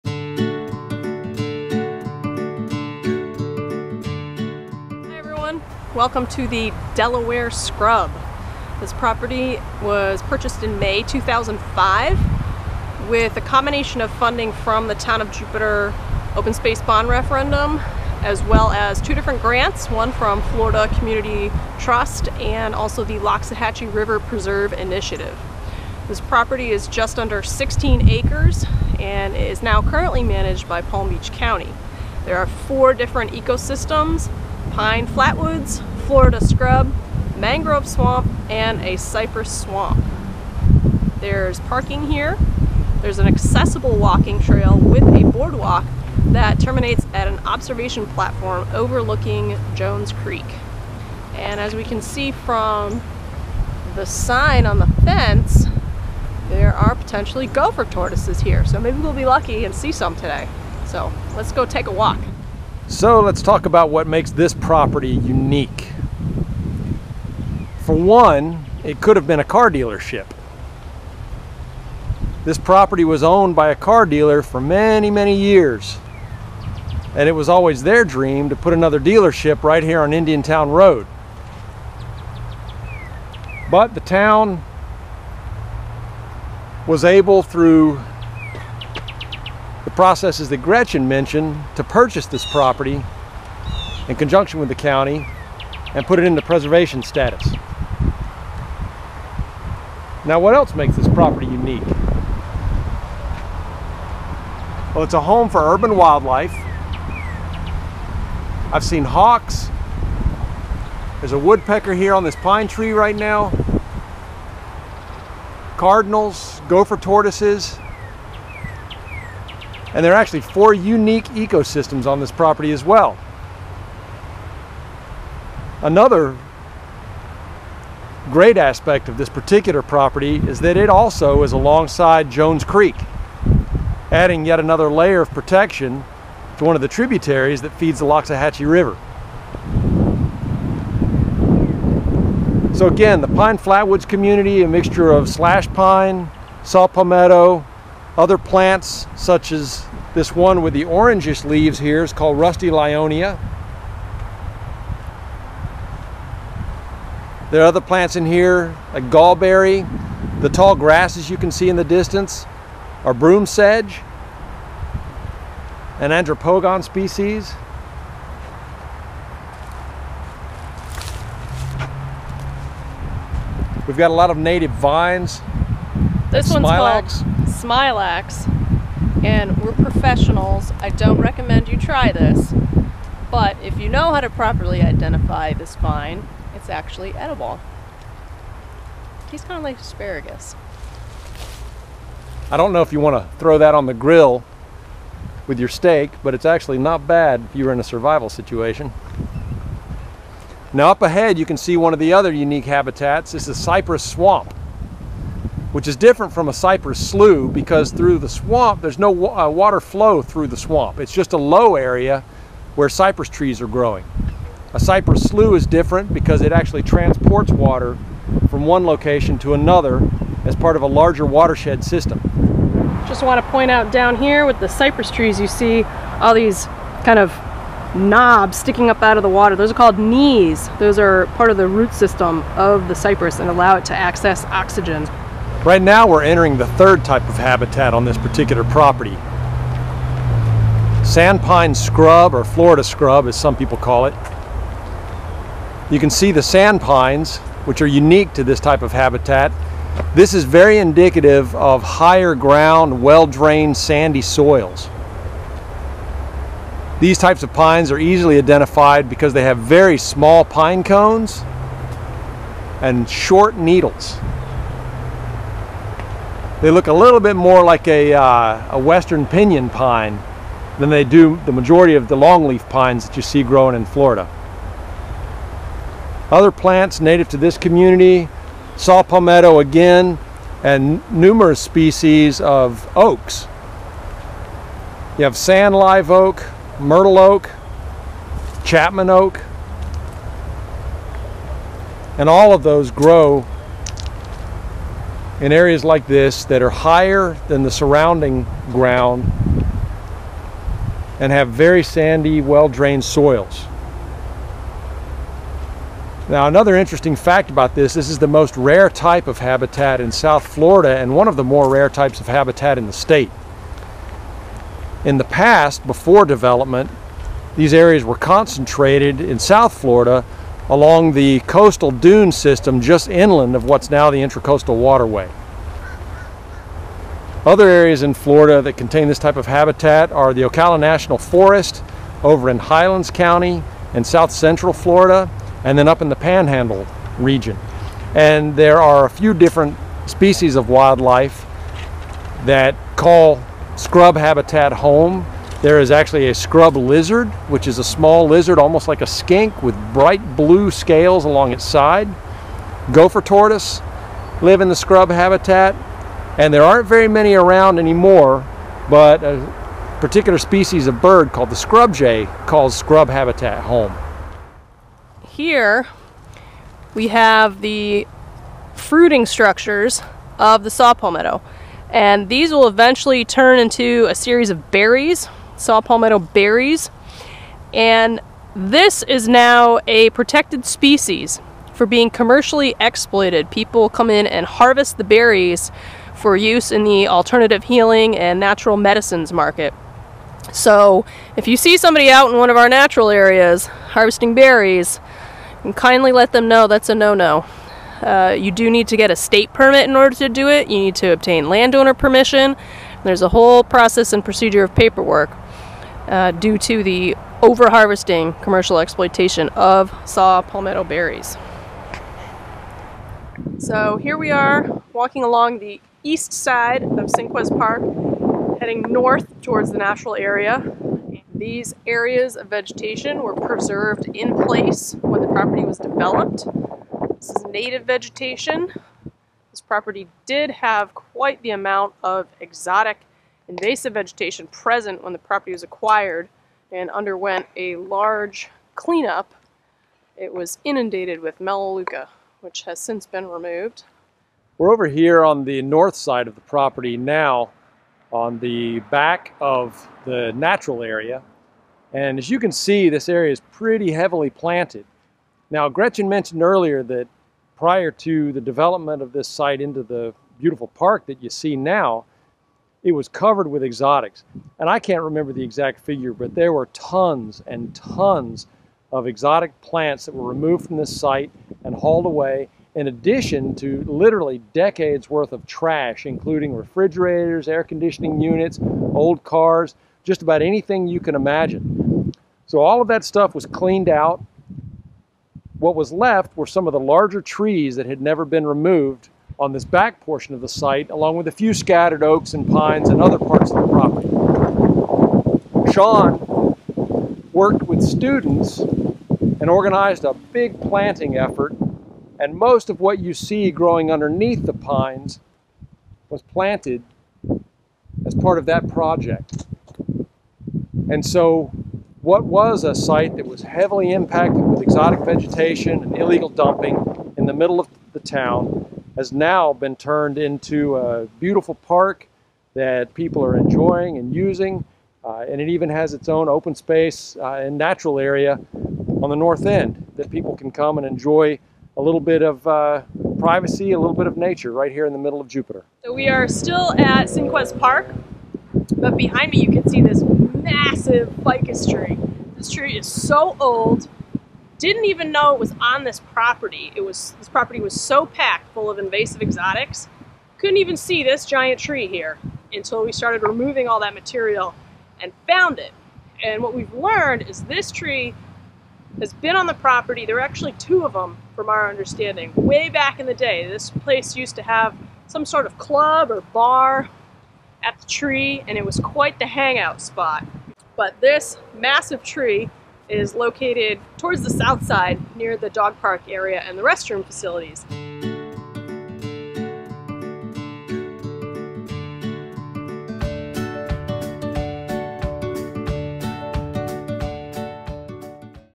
Jupiter U Open Spaces Tour Part 2
Follow along with Town Staff from the Natural Resources Division as they tour five of Jupiter's open spaces.